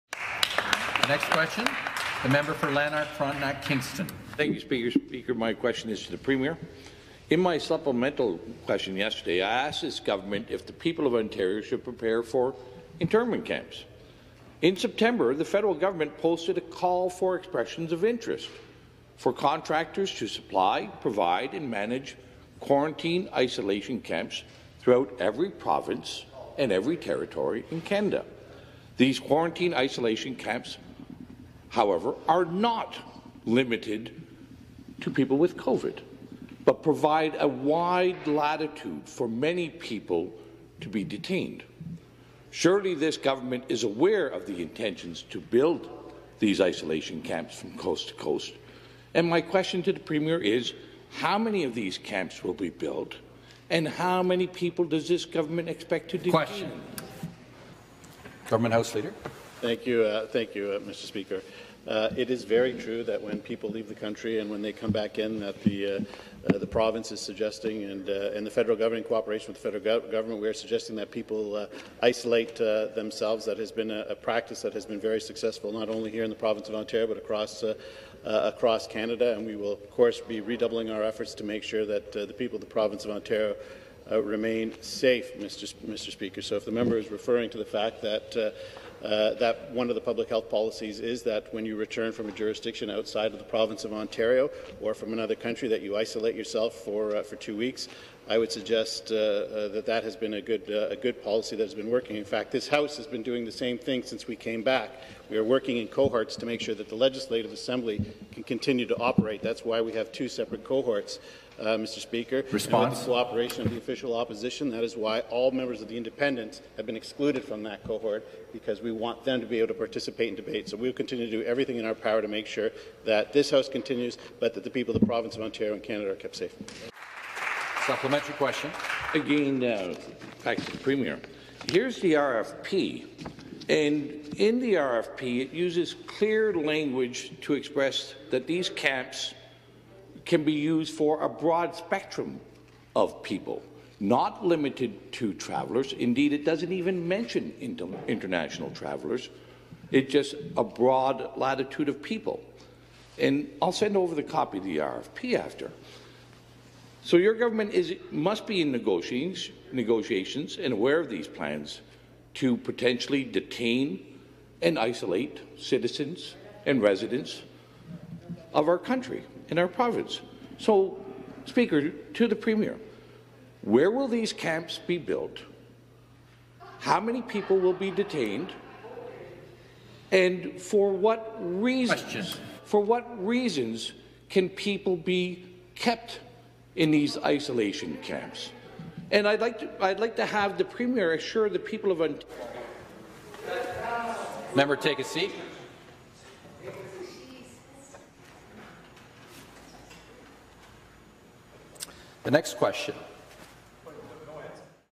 GULAGS FOR THE MASSES CANADA
internment camps in canada being discussed in Canadian parliament those who do not know history are deemed to repeat it.